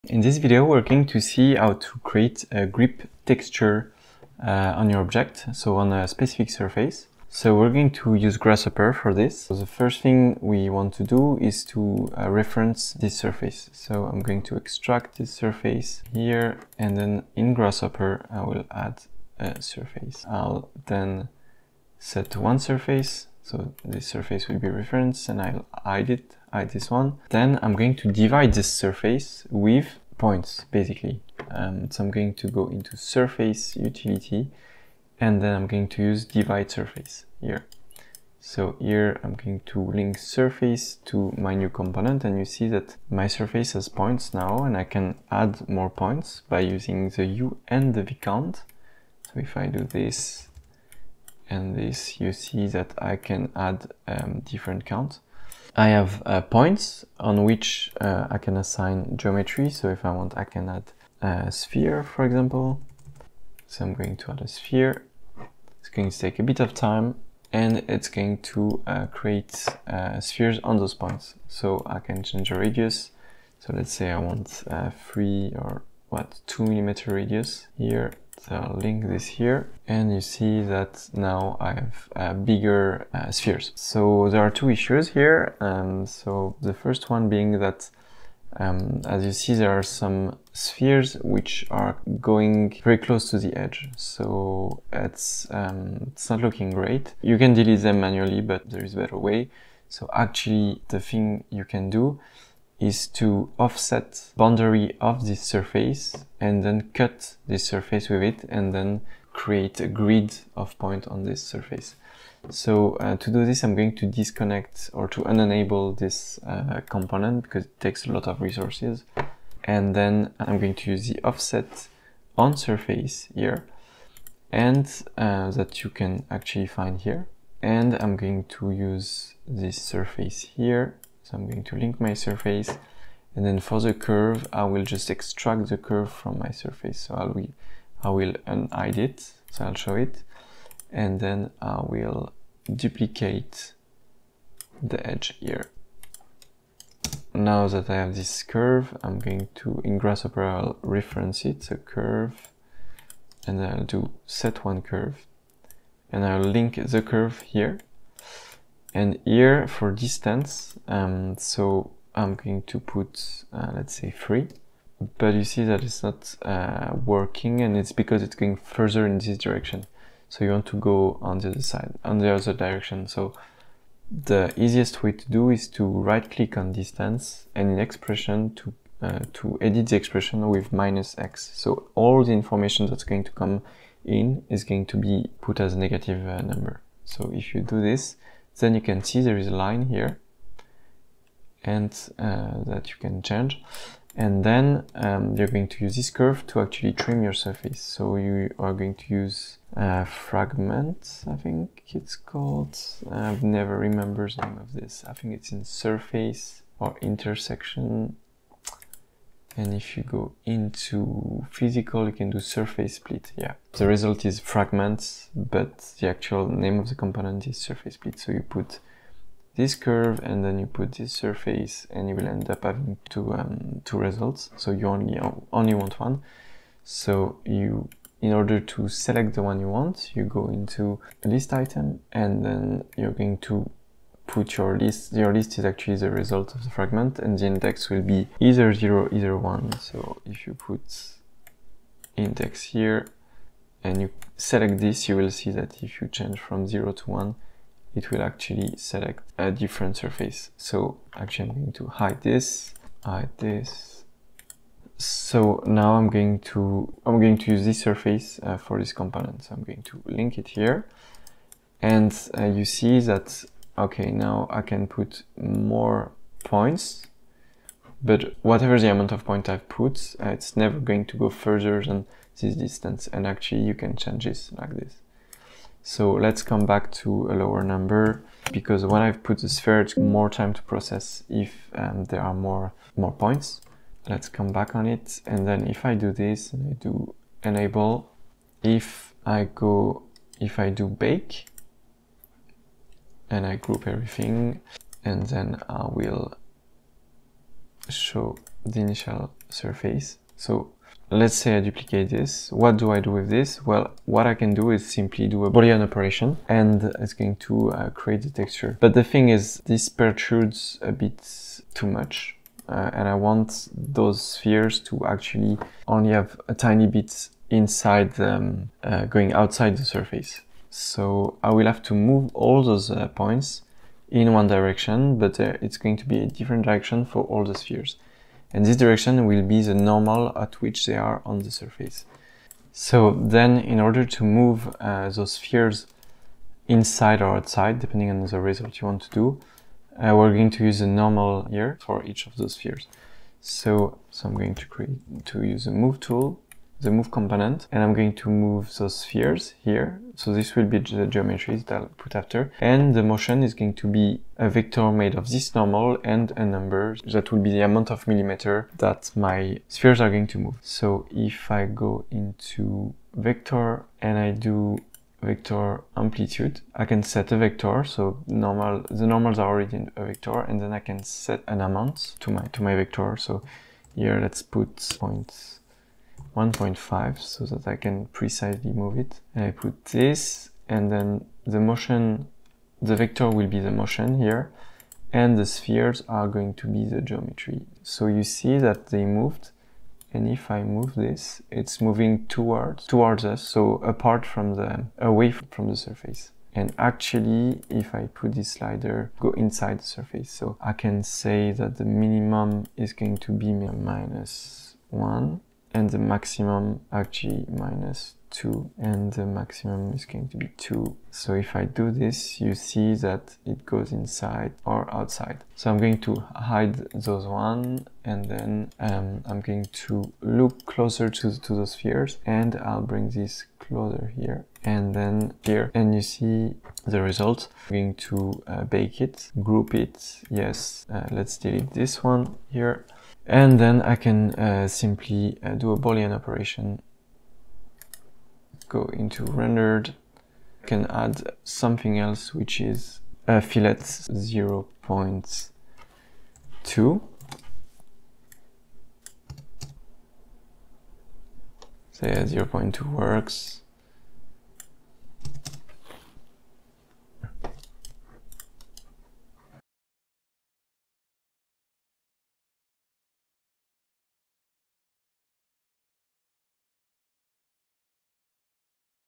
Easy grasshopper texture for product design
In this video we will see how to create a grip texture in Rhino and Grasshopper. This method can be applied to other typology of object. I will show you how to create this texture with simple component in Grasshopper.